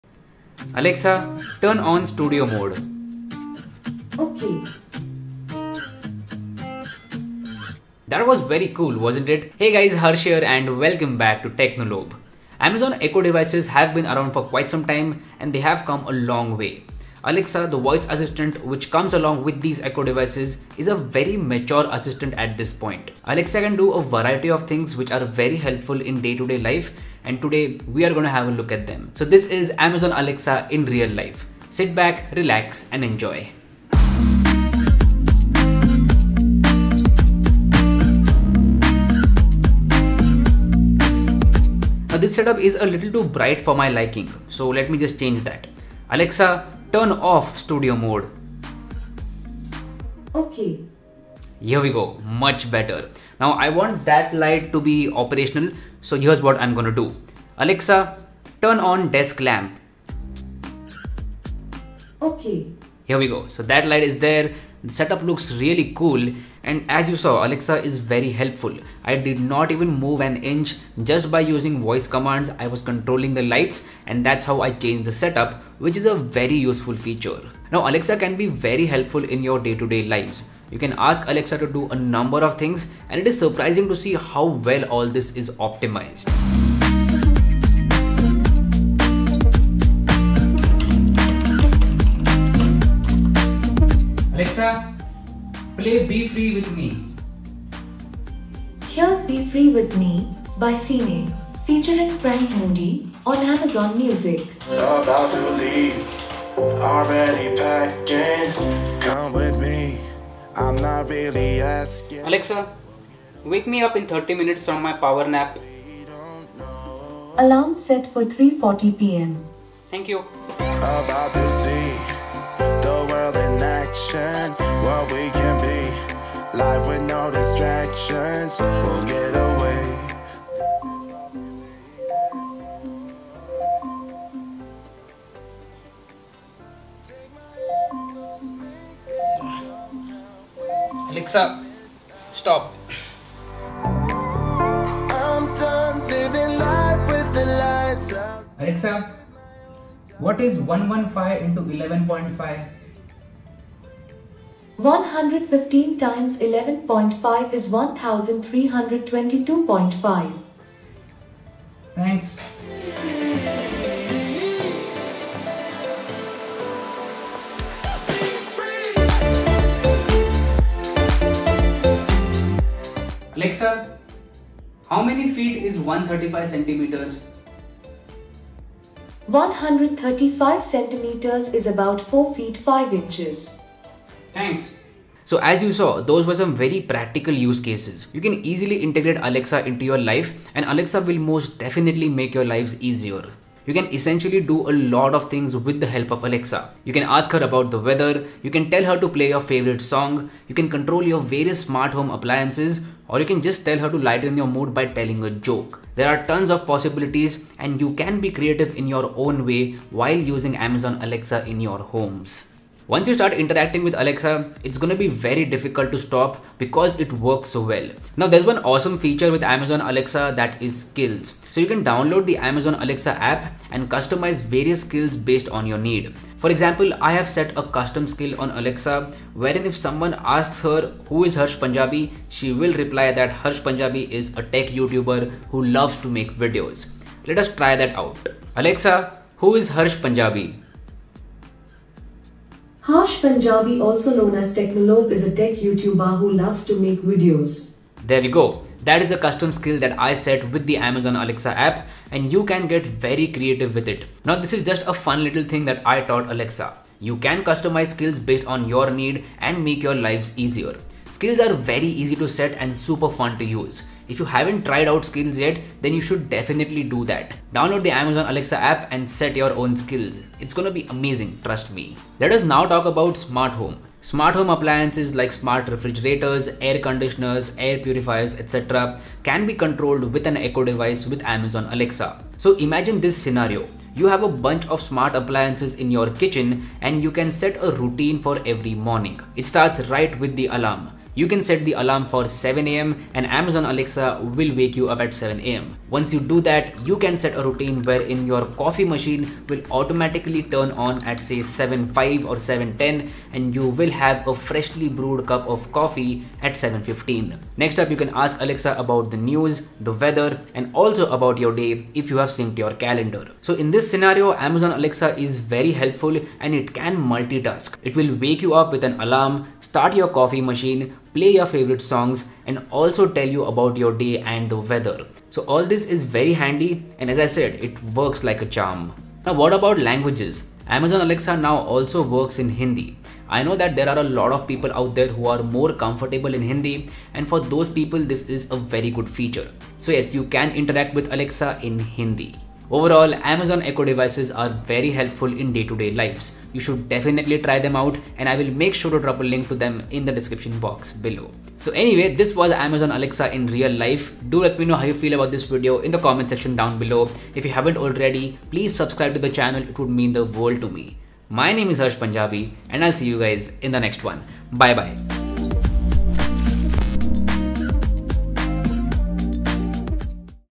Amazon Alexa in Real Life: Useful Scenarios!
Amazon Alexa has come a long way and it is a very mature virtual assistant at this point. Alexa can do a number of things and multi-task as well. Today in this video we are going to have a look at useful scenarios involving amazon Alexa and how it can make your lives easier. This is amazon Alexa in real life.